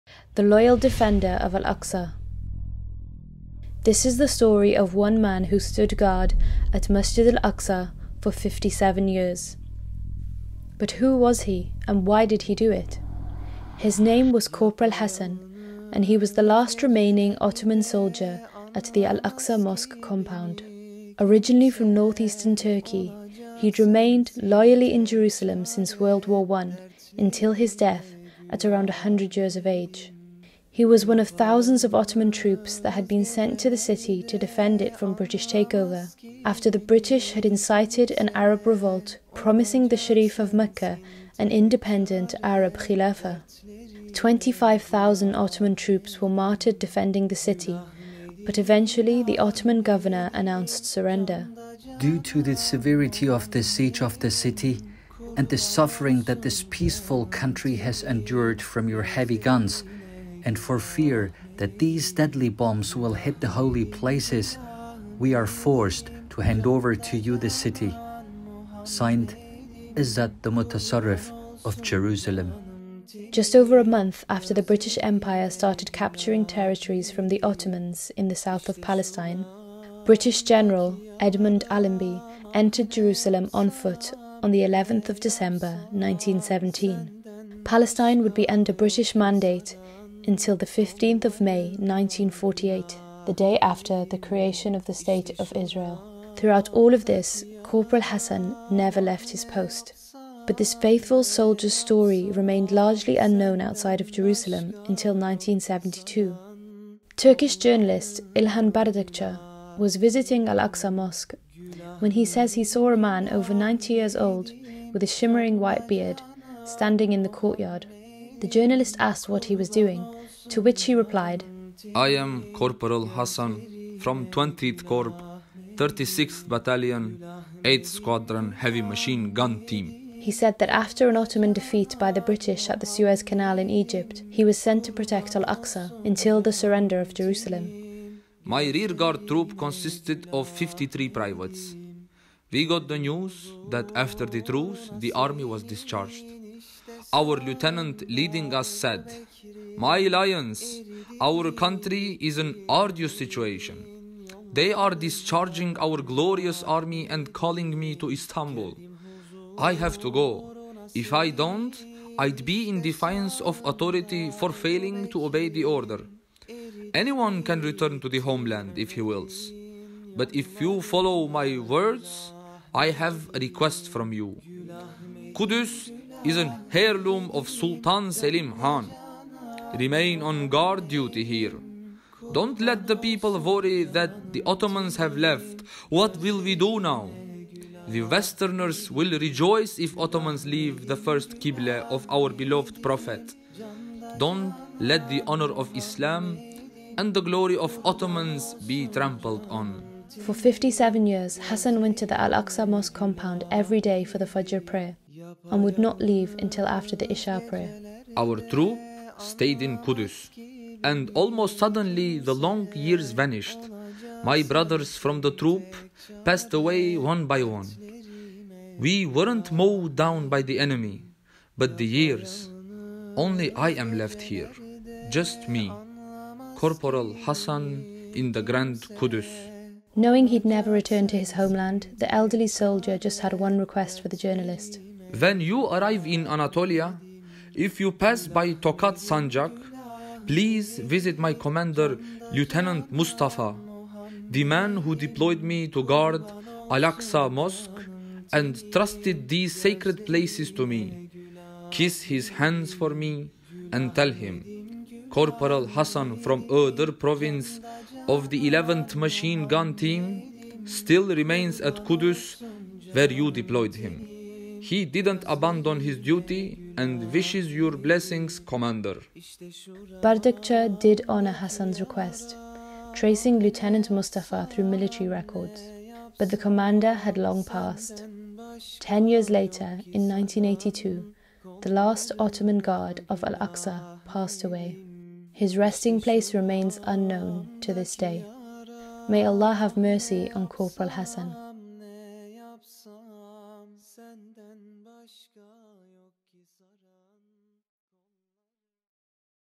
Corporal Hasan: The Loyal Defender of Al Aqsa
Corporal Hasan loyally stayed at his post from World War One until his death.

We have loads more Islamic history videos coming soon.